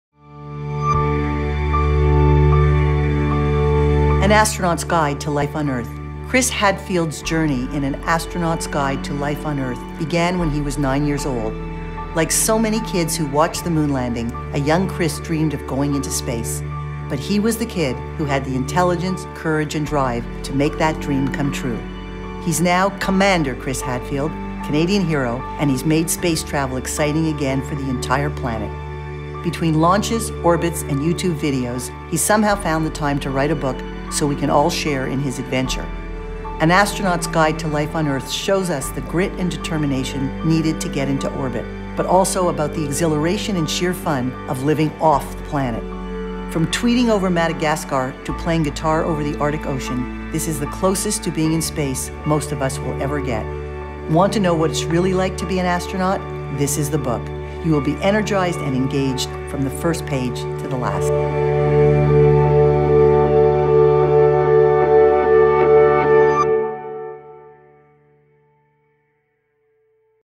Heather's Pick | An Astronaut's Guide to Life on Earth by Chris Hadfield
An Astronaut's Guide to Life on Earth by Chris Hadfield

Chris Hadfield's journey in An Astronaut's Guide to Life on Earth began when he was nine years old.  Like so many kids who watched the moon landing, a young Chris dreamed of going into space, but he was the kid who had the intelligence, courage, and drive to make that dream come true.

He's now Commander Chris Hadfield, Canadian hero, and he's made space travel exciting again for an entire planet.  And, between launches, orbits, and youtube videos, he's somehow found the time to write a book so we can all share in the adventure.  An Astronaut's Guide to Life on Earth shows us the grit and determination needed to get into orbit, but also about the exhilaration and sheer fun of living off planet.  From tweeting over Madagascar to playing guitar over the Arctic Ocean, this is the closest to being in space most of us will ever get.

Want to know what it's really like to be an astronaut? This is the book.